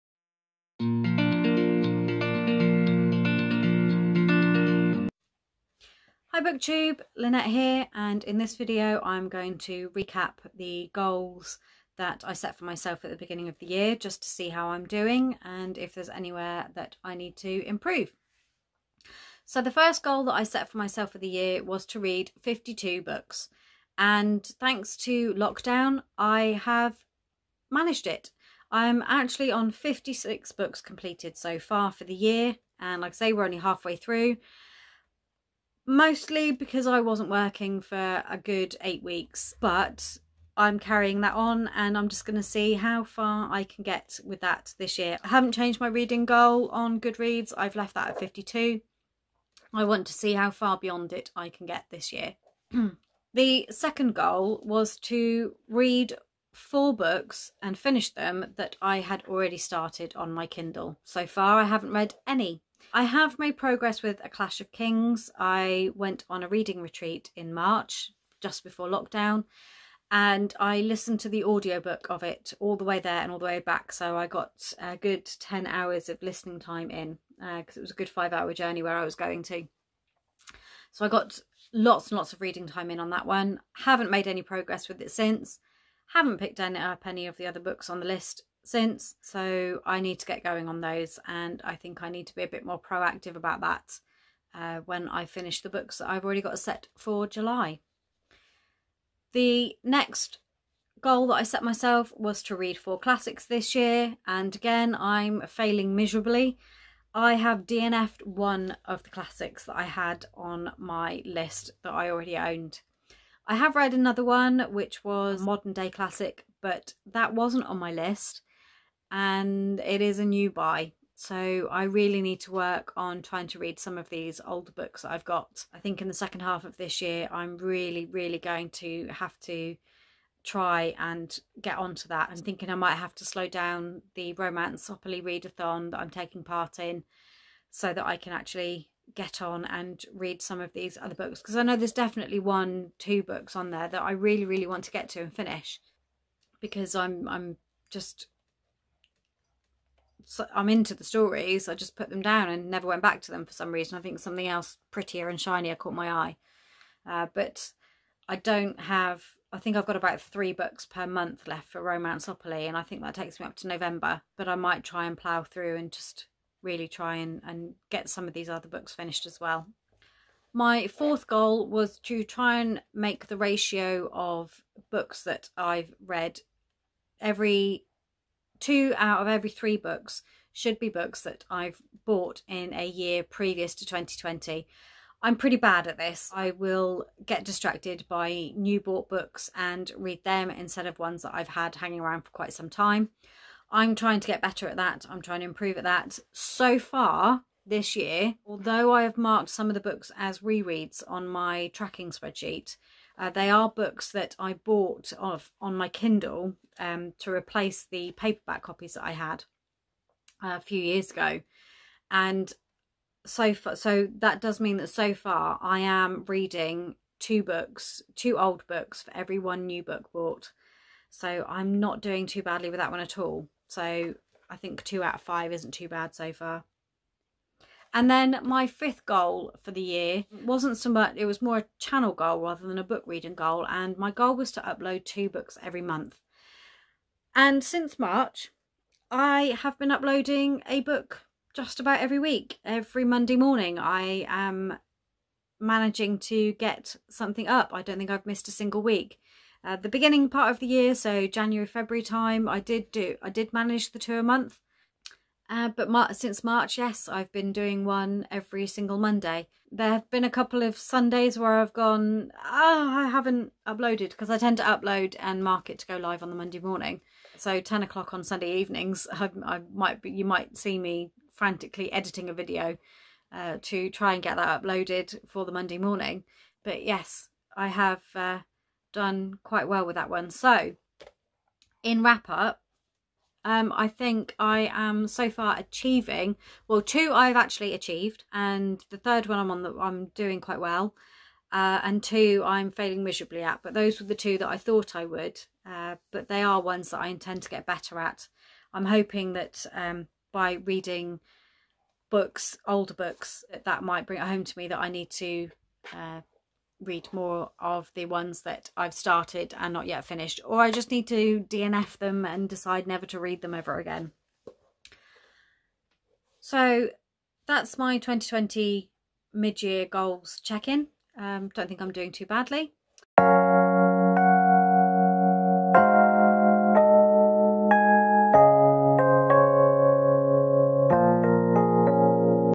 2020 Goals Check In
Hey everyone.  I decided to take a look back at the goals I set for myself at the start of the year and see how I am doing.  Some improvement needed!